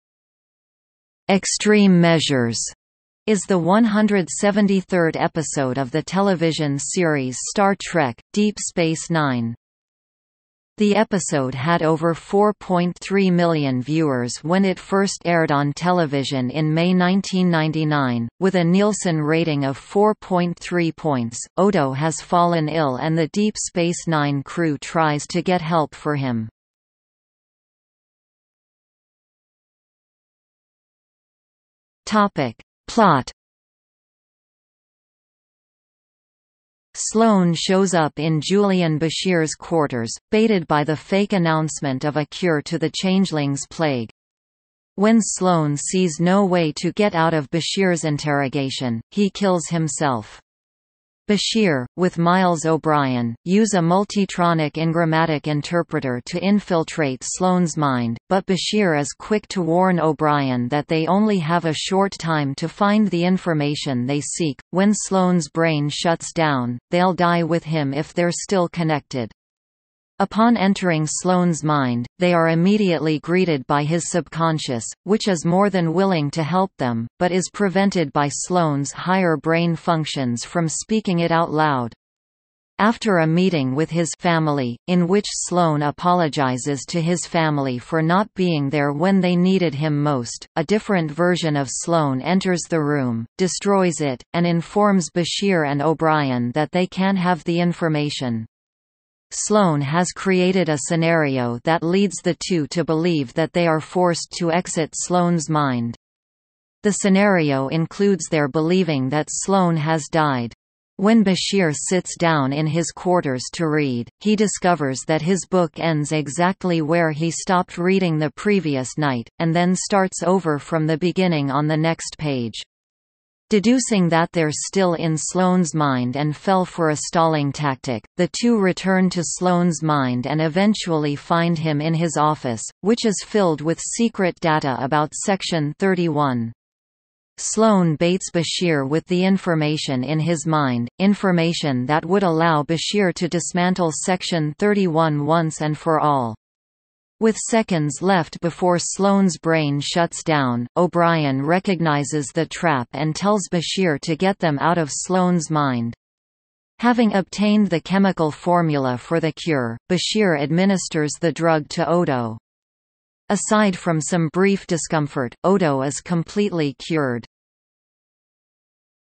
Extreme Measures (Star Trek: Deep Space Nine) | Wikipedia audio article
00:00:28 Plot

- Socrates

SUMMARY
"Extreme Measures" is the 173rd episode of the television series Star Trek: Deep Space Nine.
The episode had over 4.3 million viewers when it first aired on television in May 1999, with a Nielsen rating of 4.3 points.Odo has fallen ill and the Deep Space Nine crew tries to get help for him.